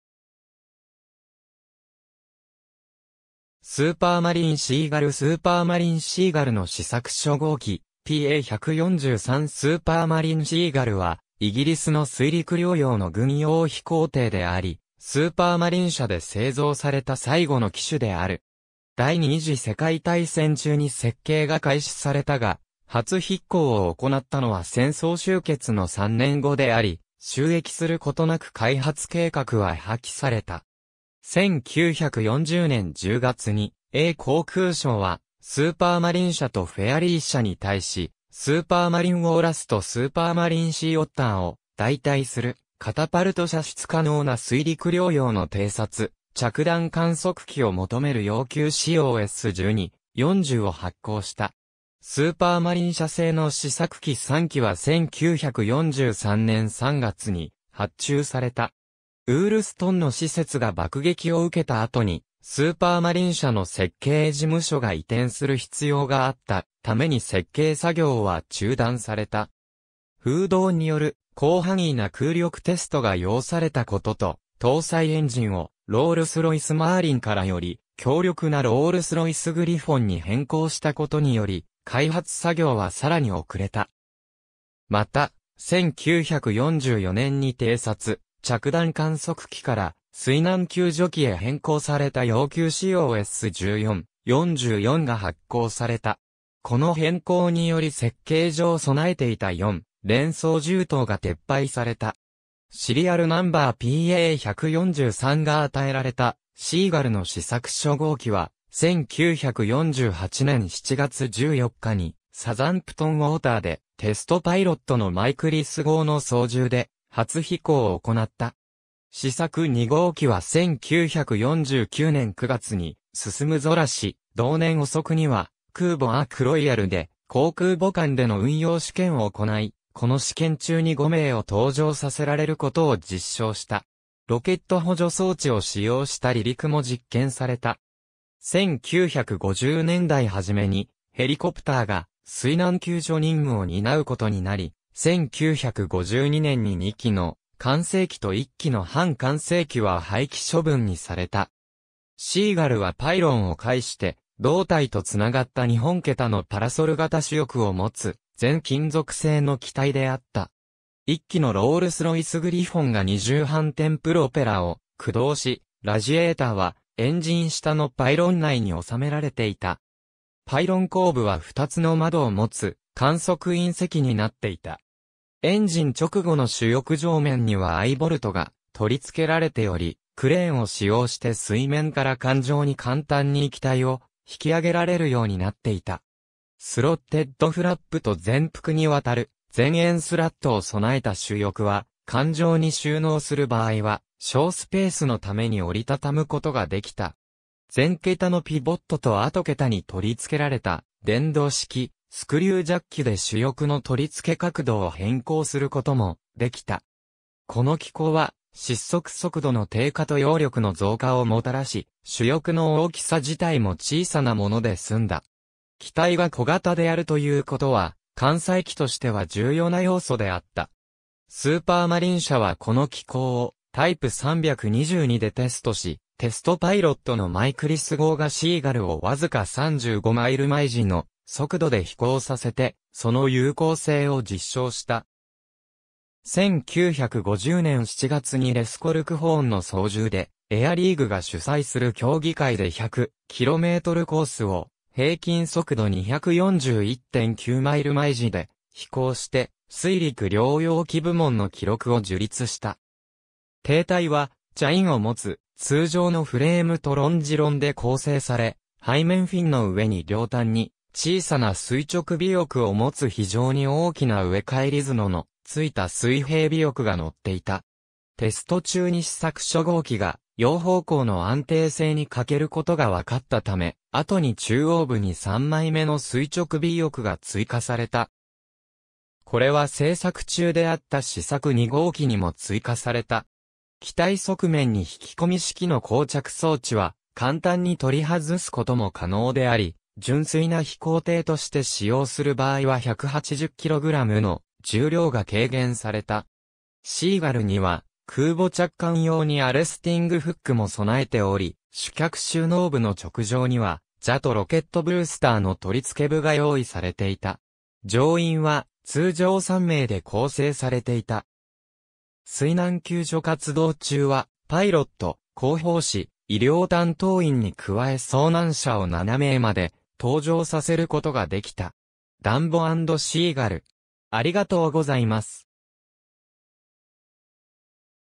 スーパーマリン シーガル (2代)
スーパーマリン シーガル スーパーマリン シーガルの試作初号機 PA143（1948年） スーパーマリン シーガル（Supermarine Seagull）は、イギリスの水陸両用の軍用飛行艇であり、スーパーマリン社で製造された最後の機種である。
第二次世界大戦中に設計が開始されたが、初飛行を行ったのは戦争終結の3年後であり、就役することなく開発計画は破棄された。
1940年10月に英航空省は、スーパーマリン社とフェアリー社に対しスーパーマリン ウォーラスとスーパーマリン シーオッターを代替するカタパルト射出可能な水陸両用の偵察/着弾観測機を求める要求仕様 S.12/40を発行した。
スーパーマリン社製の試作機3機は1943年3月に発注された。
ウールストンの施設が爆撃を受けた後にスーパーマリン社の設計事務所が移転する必要があったために設計作業は中断された。
風洞による広範囲な空力テストが要されたことと搭載エンジンをロールス・ロイス マーリンからより強力なロールス・ロイス グリフォンに変更したことにより開発作業は更に遅れた。
また、1944年に偵察/着弾観測機から水難救助機へ変更された要求仕様S.14/44（後にS.14/44/2）が発行された。
この変更により設計上備えていた4連装銃塔が撤廃された。
シリアルナンバー PA143が与えられたシーガルの試作初号機は、1948年7月14日にサザンプトン・ウォーターでテストパイロットのマイク・リスゴウの操縦で初飛行を行った。
試作2号機（PA147）は1949年9月に進空し、同年遅くには空母アーク・ロイヤルで航空母艦での運用試験を行い、この試験中に5名を搭乗させられることを実証した。
ロケット補助装置を使用した離陸も実験された。
1950年代初めにヘリコプターが水難救助任務を担うことになり、1952年に2機の完成機と1機の半完成機（PA152）は廃棄処分にされた。
シーガルはパイロンを介して胴体と繋がった2本桁のパラソル型主翼を持つ全金属性の機体であった。
1基のロールス・ロイス グリフォンが2重反転プロペラを駆動し、ラジエーターはエンジン下のパイロン内に収められていた。
パイロン後部は2つの窓を持つ観測員席になっていた。
エンジン直後の主翼上面にはアイボルトが取り付けられており、クレーンを使用して水面から艦上に簡単に機体を引き揚げられるようになっていた。
スロッテッドフラップと全幅に渡る前縁スラットを備えた主翼は、艦上に収納する場合は省スペースのために折り畳むことができた。
前桁のピボットと後桁に取り付けられた電動式スクリュージャッキで主翼の取り付け角度を変更することもできた。
この機構は失速速度の低下と揚力の増加をもたらし、主翼の大きさ自体も小さなもので済んだ。
機体が小型であるということは艦載機としては重要な要素であった。
スーパーマリン社はこの機構をType 322でテストし、テストパイロットのマイク・リスゴウがシーガルを僅か35マイル毎時 (56 km/h)の速度で飛行させてその有効性を実証した。
1950年7月にレス・コルクホーンの操縦でエアリーグが主催する競技会で100-キロメートル (62 mi)コースを平均速度241.9マイル毎時 (389.3 km/h)で飛行して水陸両用機部門の記録を樹立した。
艇体はチャインを持つ通常のフレームとロンジロンで構成され、背面フィンの上に両端に小さな垂直尾翼を持つ非常に大きな上反角のついた水平尾翼が乗っていた。
テスト中に試作初号機がヨー方向の安定性に欠けることが分かったため、後に中央部に3枚目の垂直尾翼が追加された。
これは製作中であった試作2号機にも追加された。
機体側面に引き込み式の降着装置は簡単に取り外すことも可能であり、純粋な飛行艇として使用する場合は180キログラム (400 lb)の重量が軽減された。
シーガルには空母着艦用にアレスティング・フックも備えており、主脚収納部の直上にはJATOロケットブースターの取り付け部が用意されていた。
乗員は通常3名で構成されていた。
水難救助活動中はパイロット、航法士、医療担当員に加え遭難者を7名まで搭乗させることができた。
(シーガル ASR 1 の性能は出力2,500 hpのグリフォン RG30SM搭載時の計画値) Dumbo and Seagull